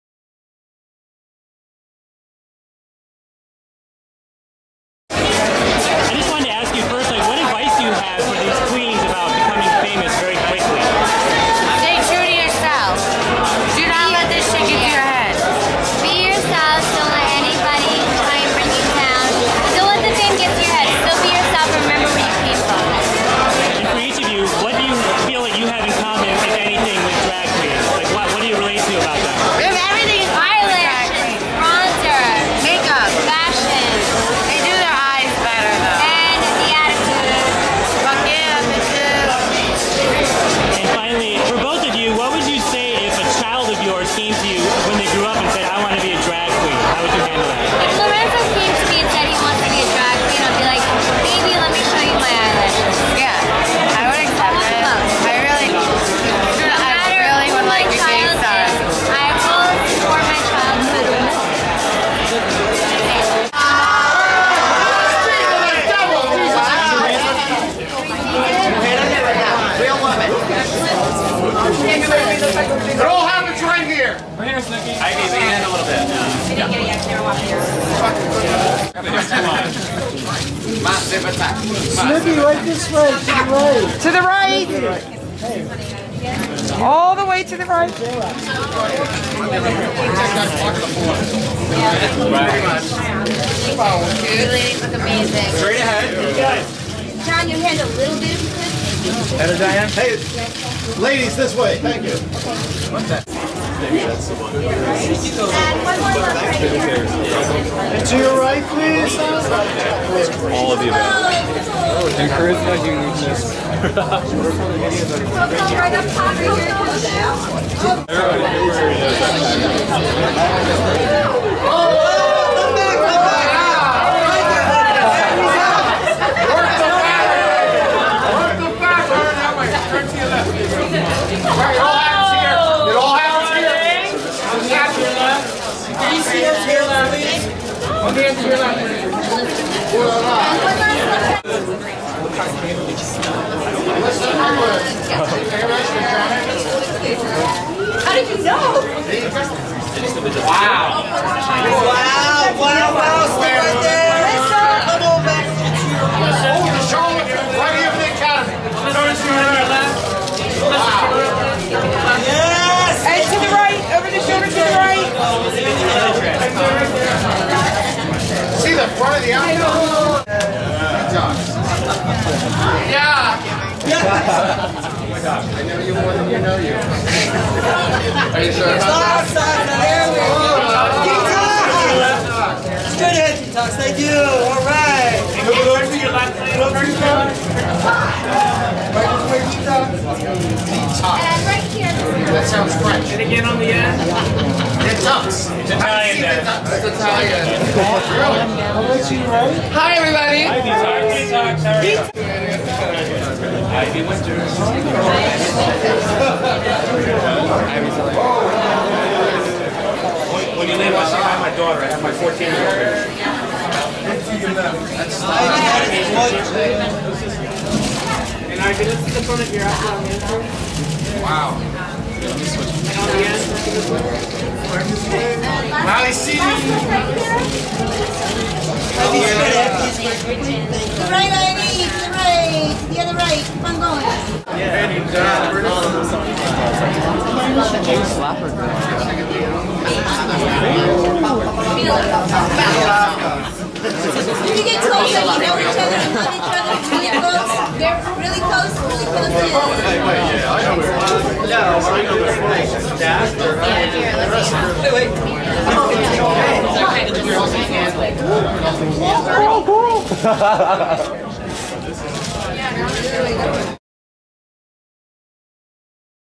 SNOOKI & Busty JWOWW: How They'd React to a Drag Queen Son!
BoyCulture.com interviews Snooki & JWoww at XL in NYC on 1.25.13 about why they relate to drag queens and how they'd react to a drag queen as a son.

Also, there's some fun red-carpet footage of the girls from Season 5 of RuPAUL'S DRAG RACE on the red carpet.

(This last one is Work Unfriendly)